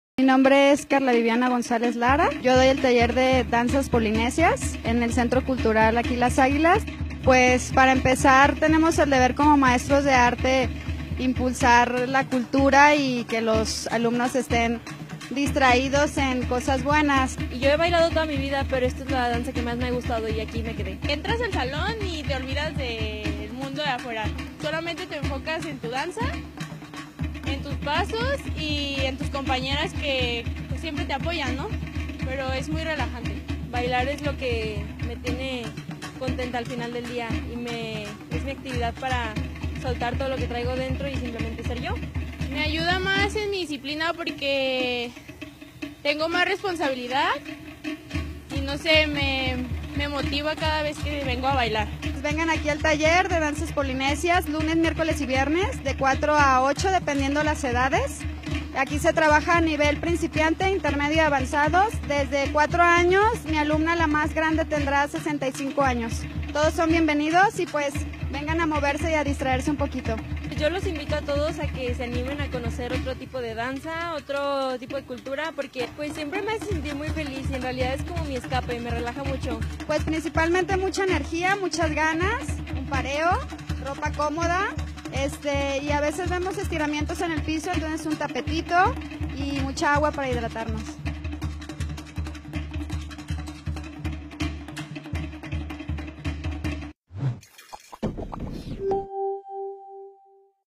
Centros Culturales en el municipio - Gobierno de Zapopan
¿Sabías que en Zapopan contamos con 6 Centros Culturales?

Danzas polinesias es uno de los más de 100 talleres que tenemos en CulturaZapopan y que ponemos a tu disposición. Te compartimos algunas experiencias de nuestros alumnos y profesora del Centro Cultural Las Águilas, donde muestran un poco de su trabajo y su sentir al practicar esta disciplina artística.

Mayores informes al 38182200 ext. 3852

Visita nuestro sitio web y entérate de todos los detalles También puedes seguirnos en Facebook y Twitter